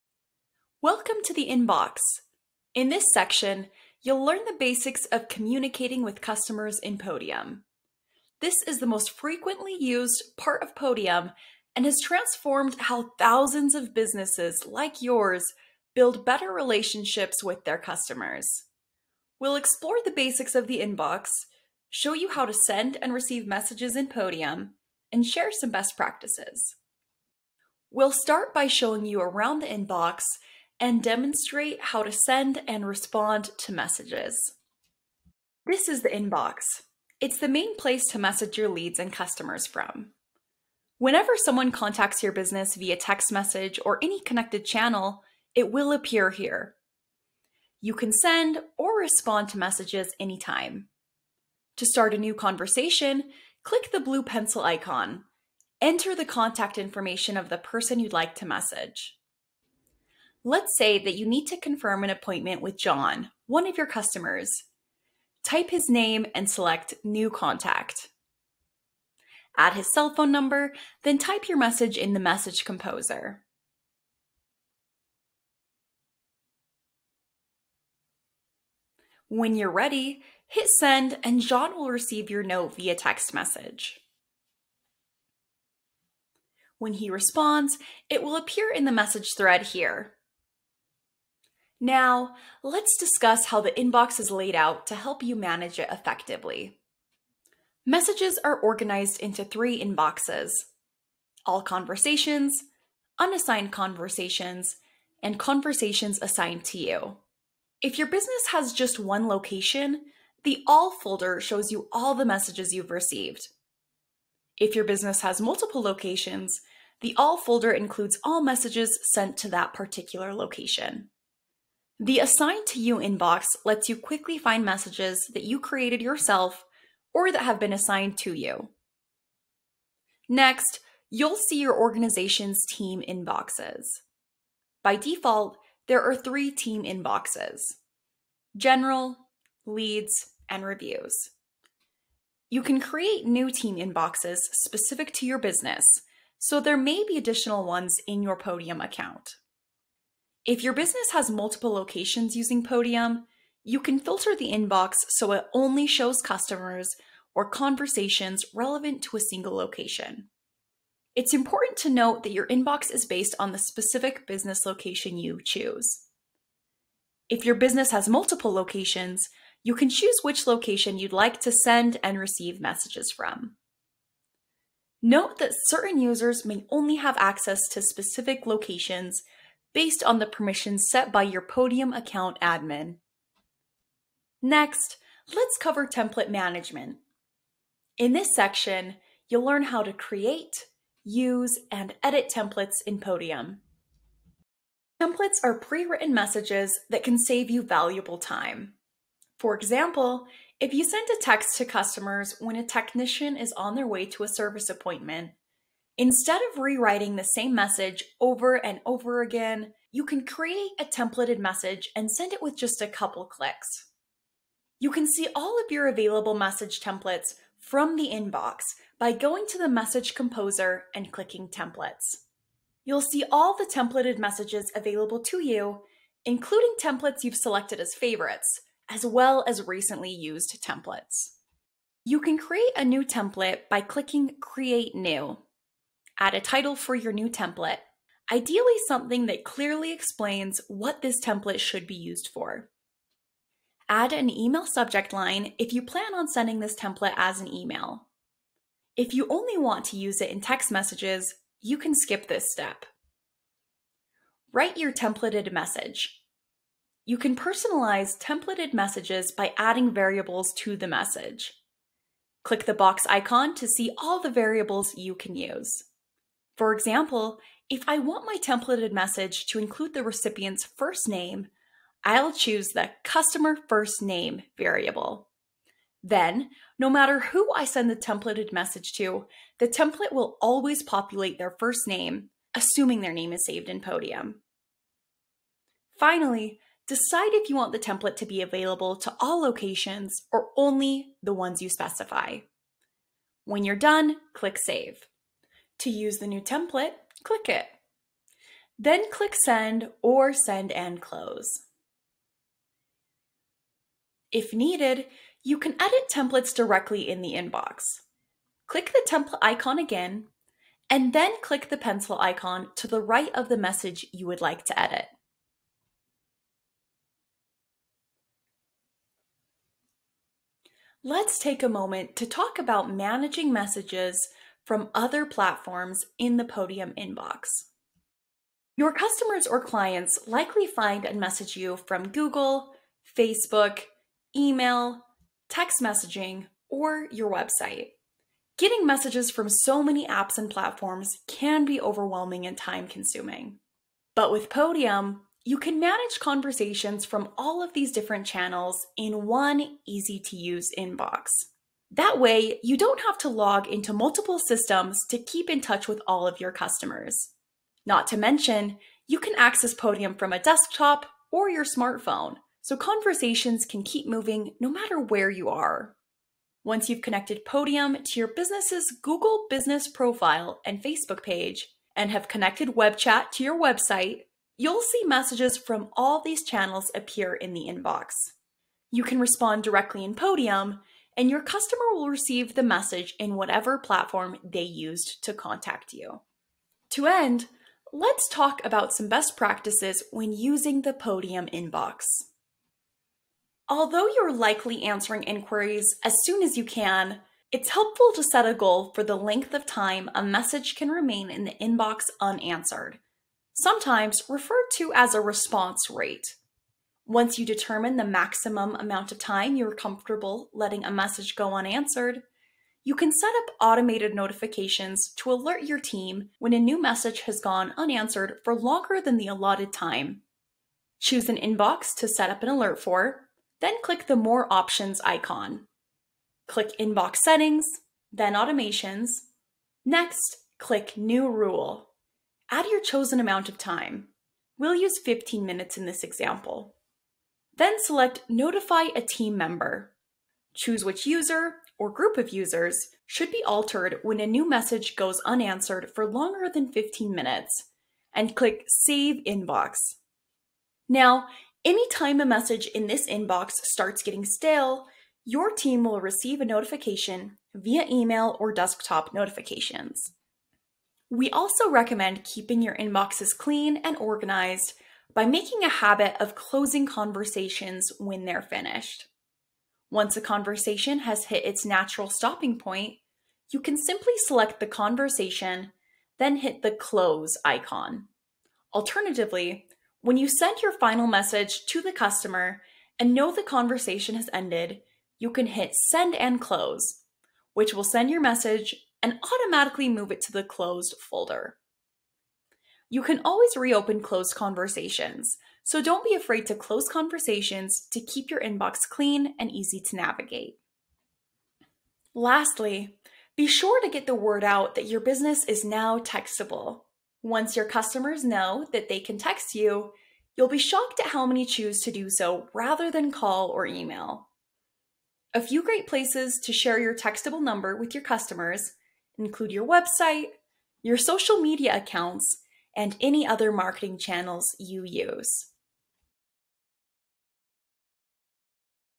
Using the Podium Inbox
In this tutorial, you'll learn how to use the Podium Inbox to send & receive messages, organize your conversations, and other Inbox best practices.